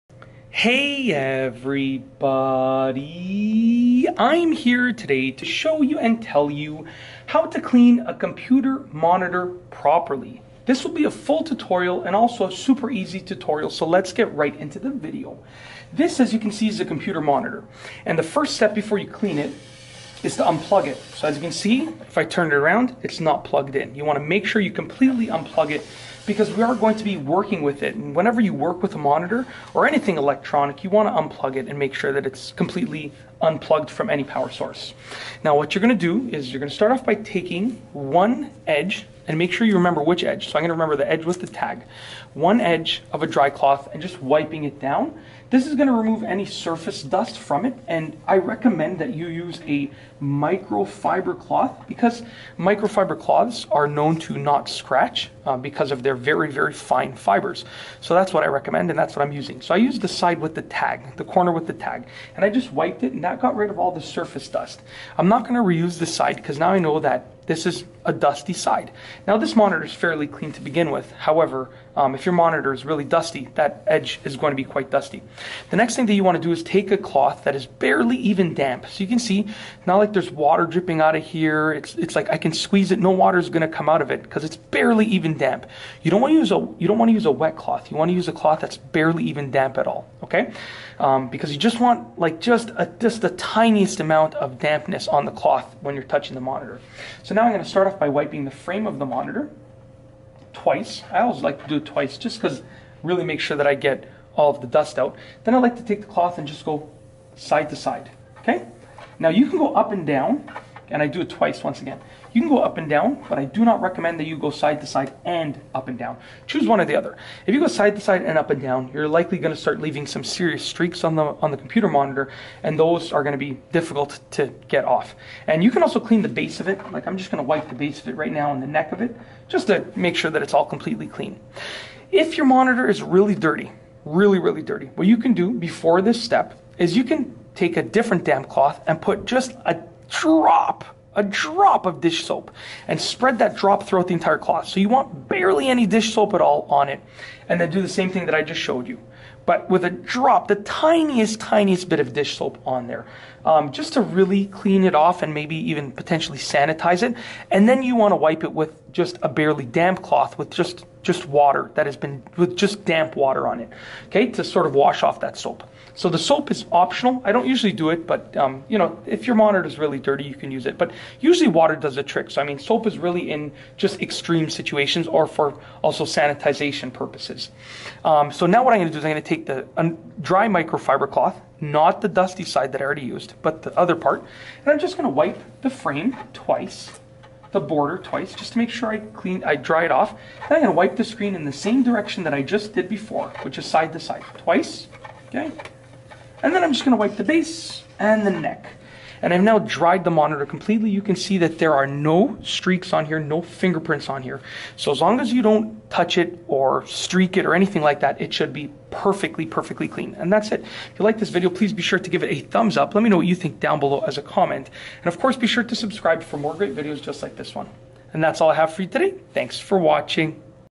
How To Clean A Computer Monitor PROPERLY-Full Tutorial
In this video, I show you how to clean a computer monitor properly. If your computer monitor has fingerprints and streaks all over it, then this is the video for you. I go over a simple method that you can use to clean it. The best thing about this method is that it doesn't use any special tools or products, it just makes use of a microfiber cloth that you probably have lying around your house anyways. It leaves the computer monitor absolutely spotless and eliminates the glare that fingerprints cause substantially!